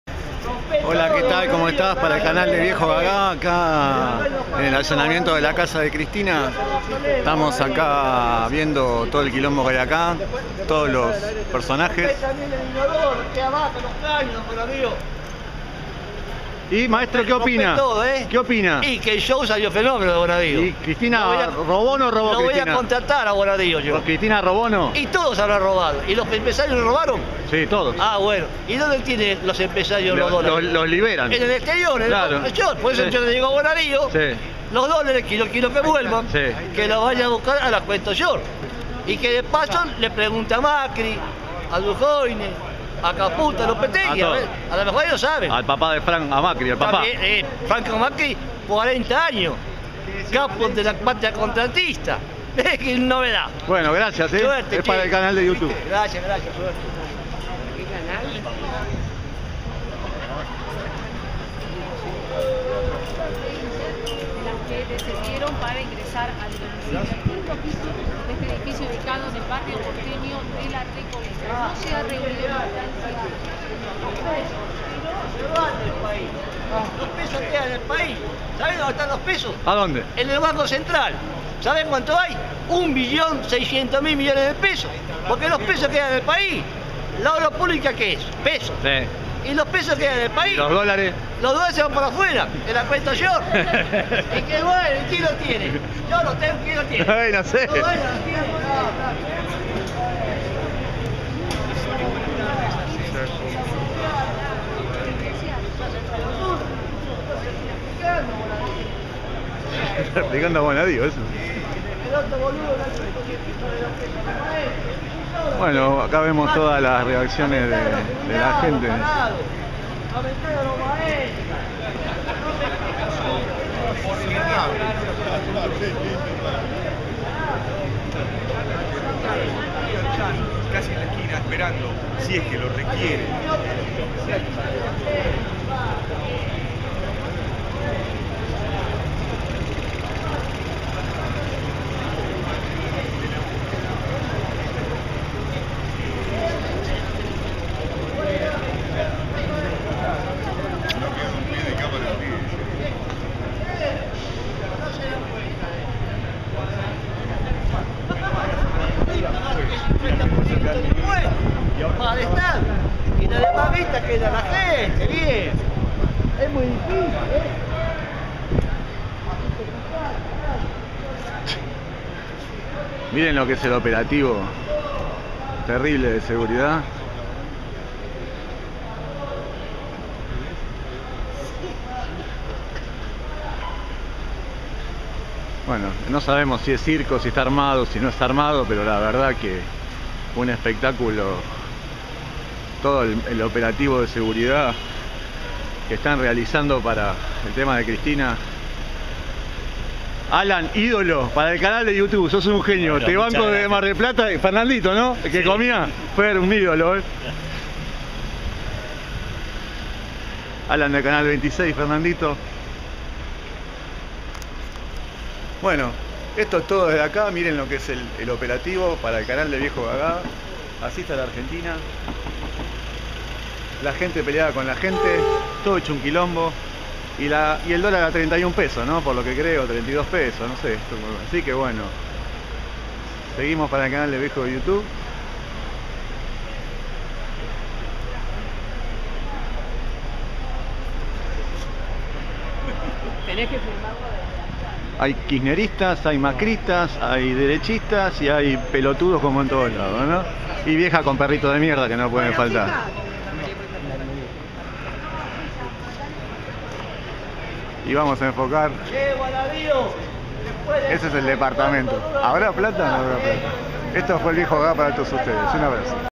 Allanamiento a CFK ,lo que ningún medio te mostro, VIEJOGAGA VIDEOS
Allanamiento en la casa de de Cristina Kirchner , Calle Uruguay 1309 CABA , gente agresiva , macrista , choriplaneros y vagos de todo tipo , así esta el país . Filmas y te viene a molestar diciendo que sos del servicio secreto.